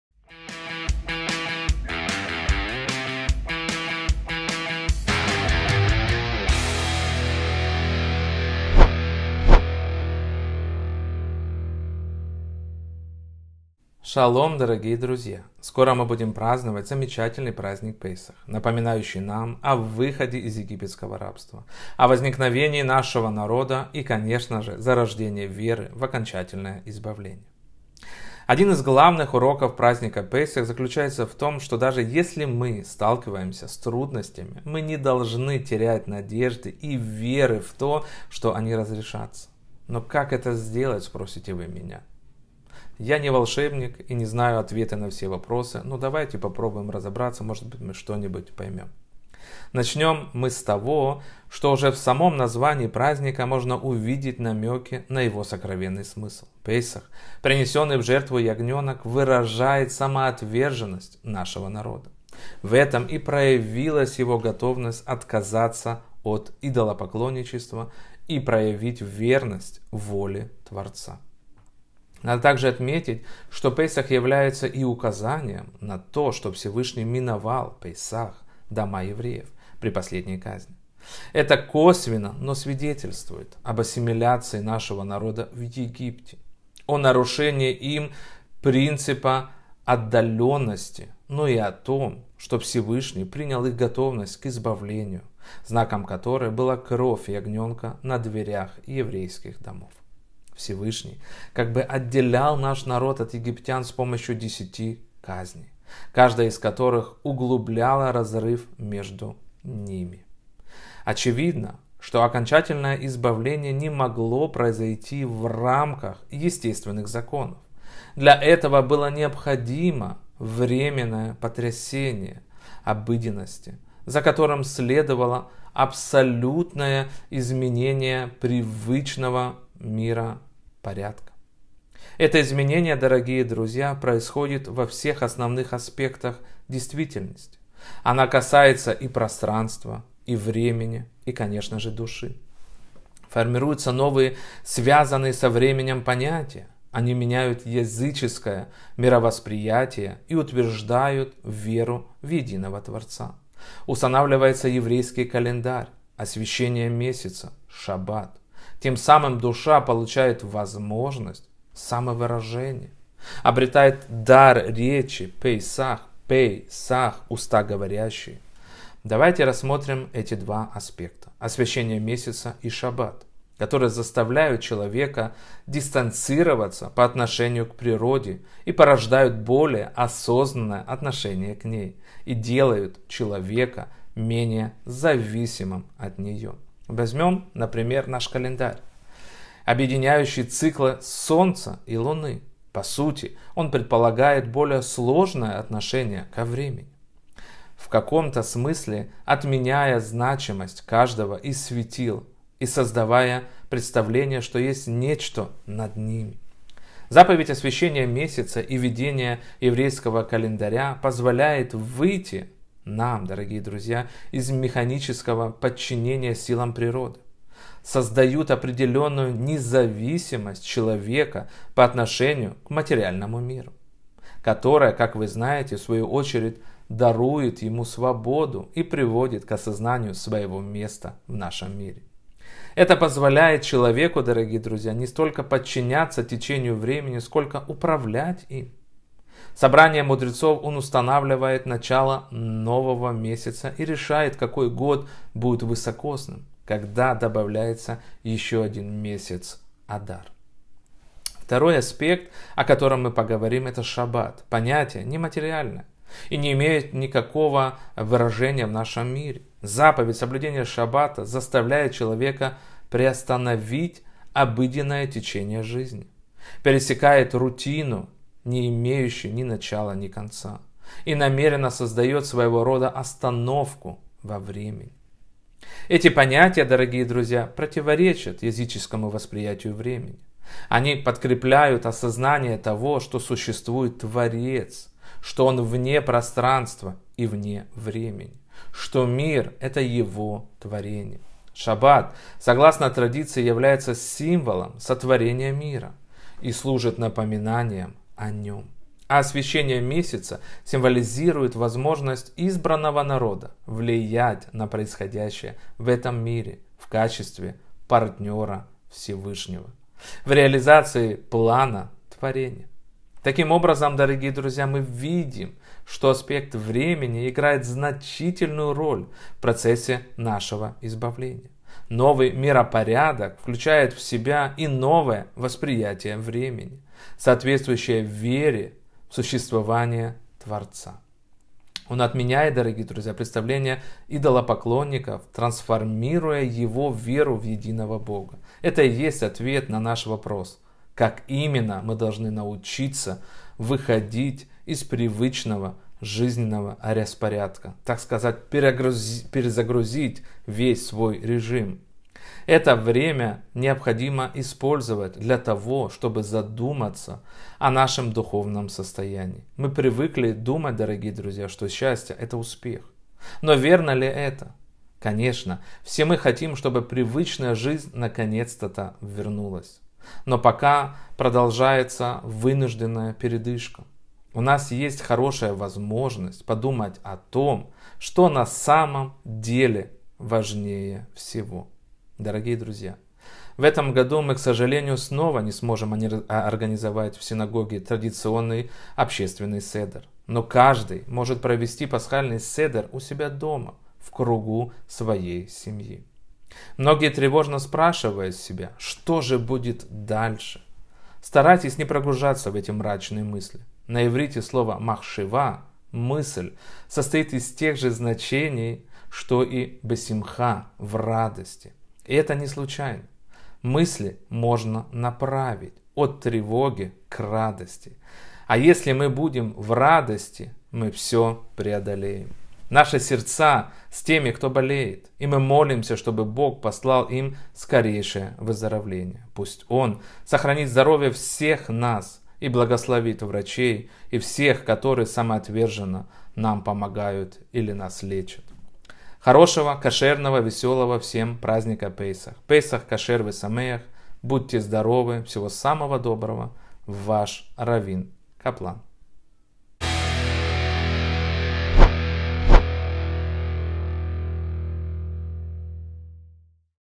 Песах: Духовная перезагрузка | Размышления на тему Праздников
Дорогие друзья! Шалом!
Скоро мы будем праздновать Песах, напоминающий нам о выходе из египетского рабства, о возникновении нашего народа и зарождении веры в окончательное избавление. Один из главных уроков Песах заключается в том, что даже если мы сталкиваемся с трудностями, мы не должны терять надежды и веры в то, что они разрешатся. «Но как это сделать?» - спросите вы меня.
Давайте попробуем разобраться.